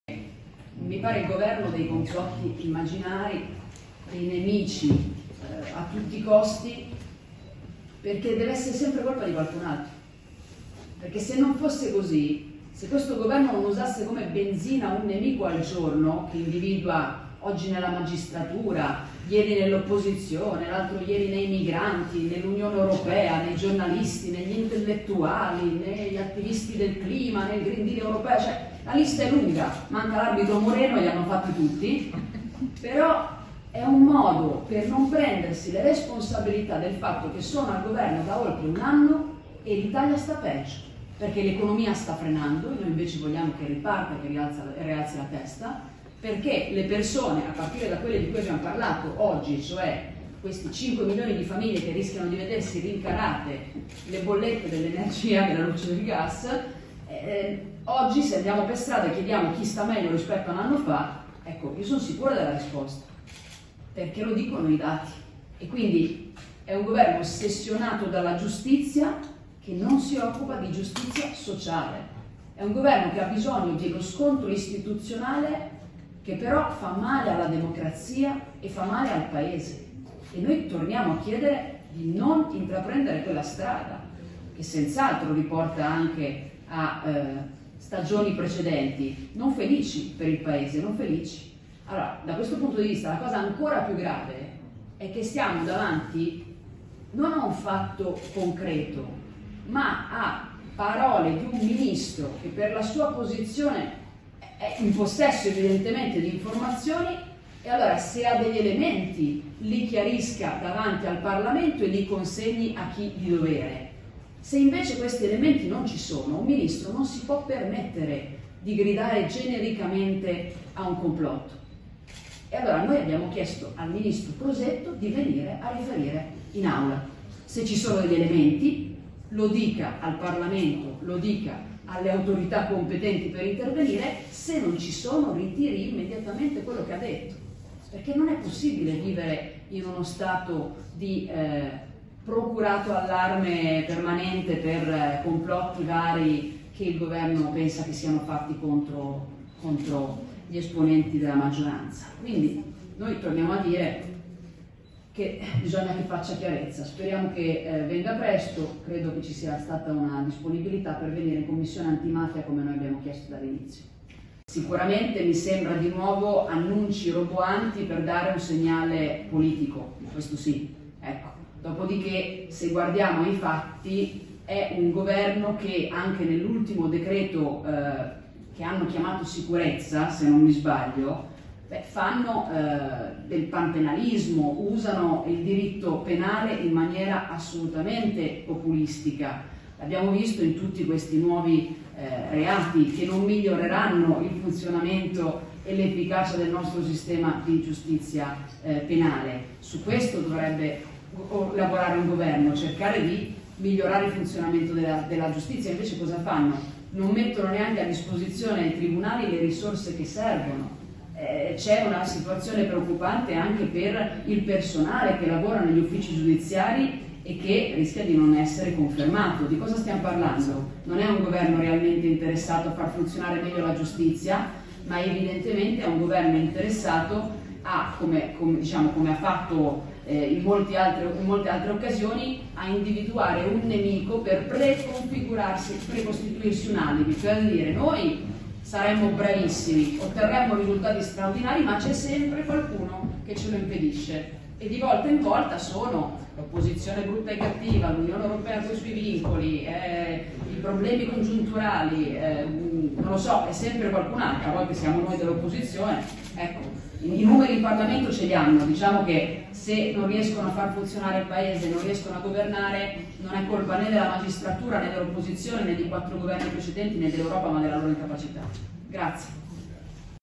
Giustizia e caso Crosetto, Elly Schlein attacca il governo
Giustizia, le parole della segretaria Pd Elly Schlein